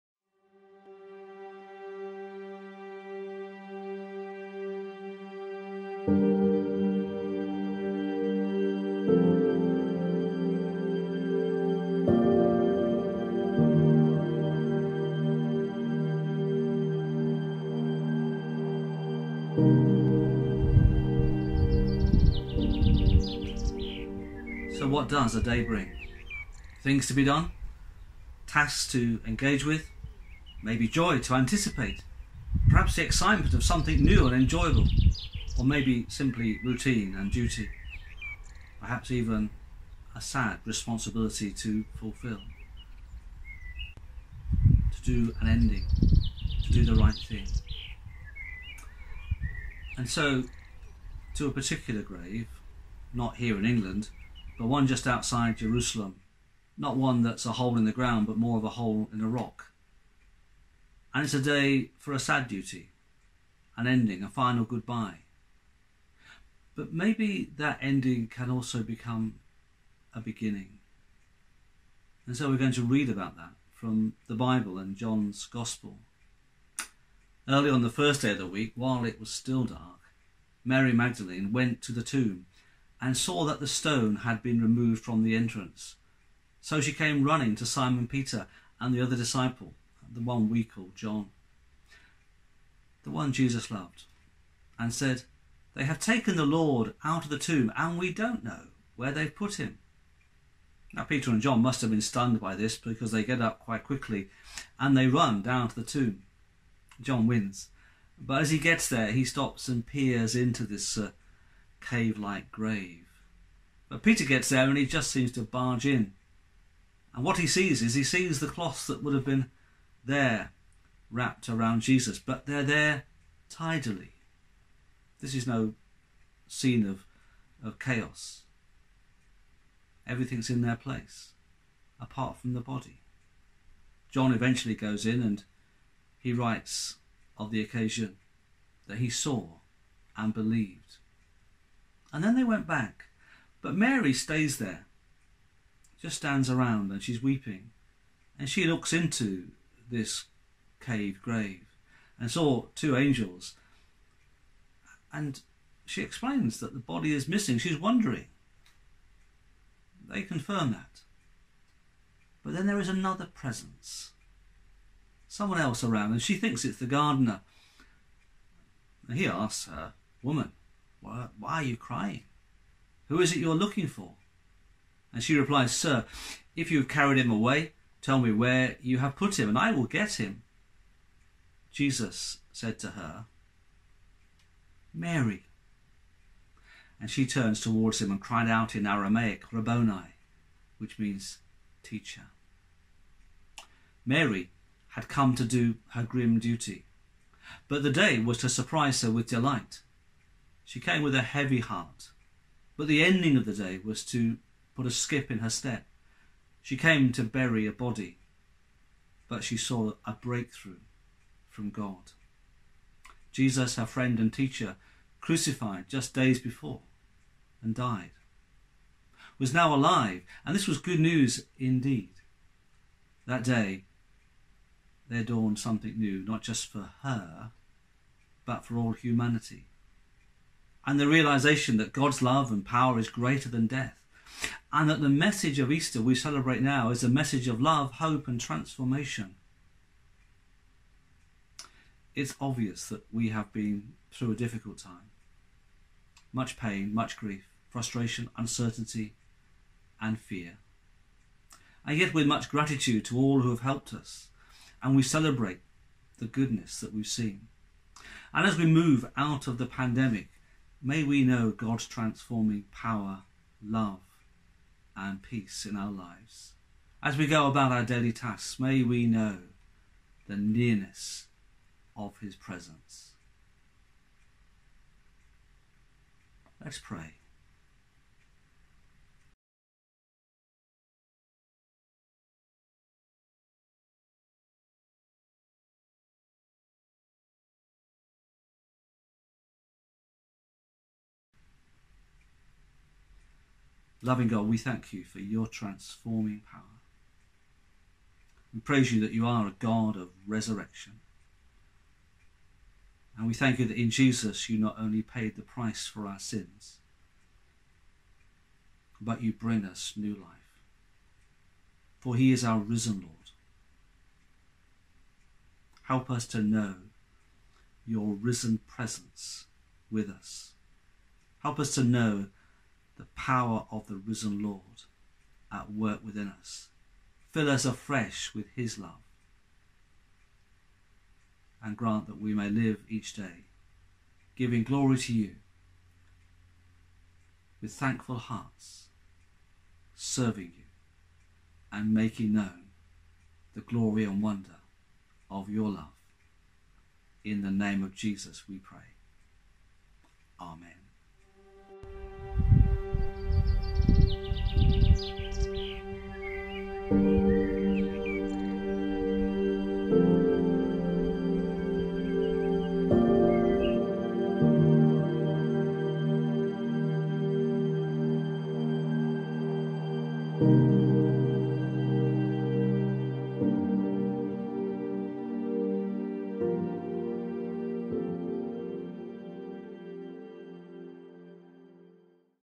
Journeying with Jesus - Holy Week reflection for Easter Day
John Claydon reflects on the empty tomb (John 20:1-18)